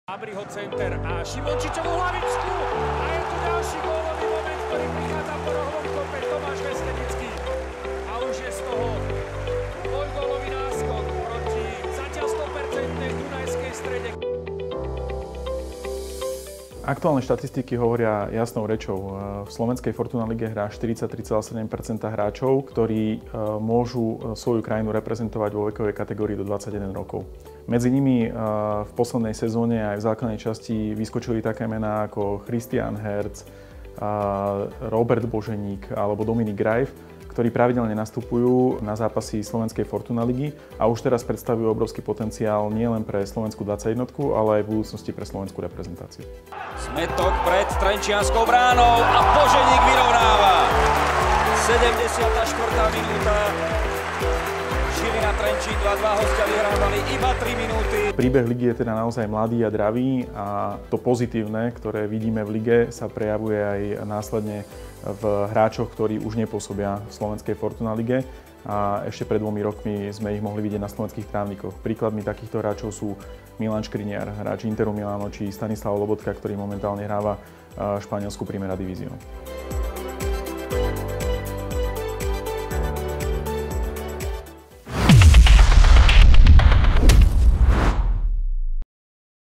Fortuna liga = liga talentov
Štatistiky hovoria jasnou rečou. V slovenskej Fortuna lige je 43,7% hráčov, ktorí môžu reprezentovať svoju krajinu vo vekovej kategócii do 21 rokov! Fortuna liga je mladá, dravá, plná talentovaných futbalistov. Viac hovorí výkonný riaditeľ ÚLK Michal Mertinyák!